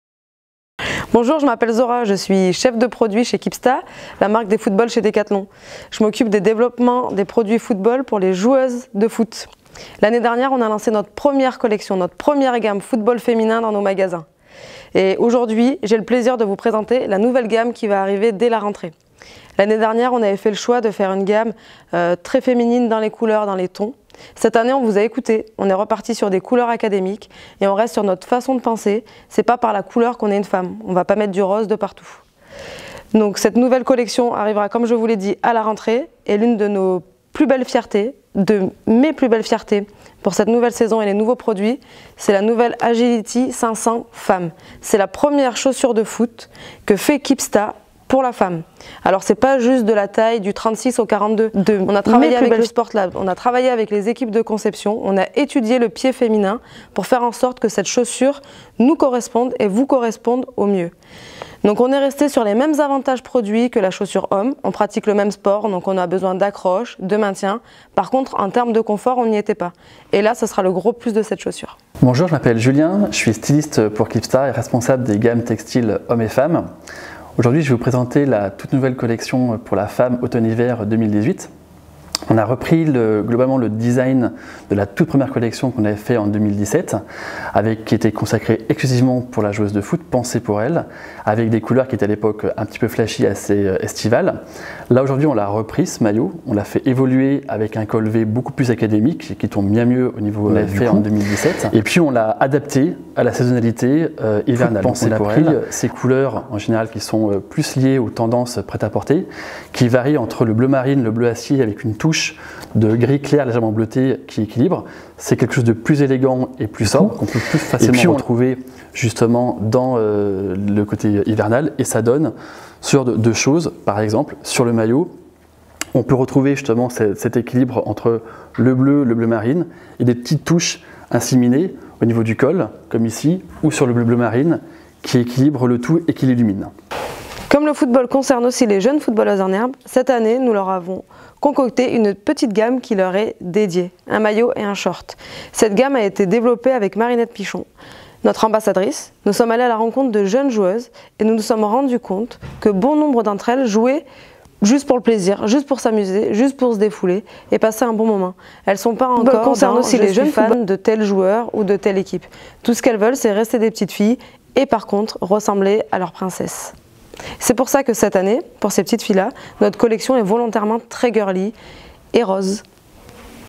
Get behind the scenes of the new Kipsta Women's Football range by Decathlon
Zohra, Product Manager for Women's Football and Julien, Textile Designer at Kipsta explain their roles in the design and conception of the new textile football range dedicated to women footballers.
To discover as soon as possible!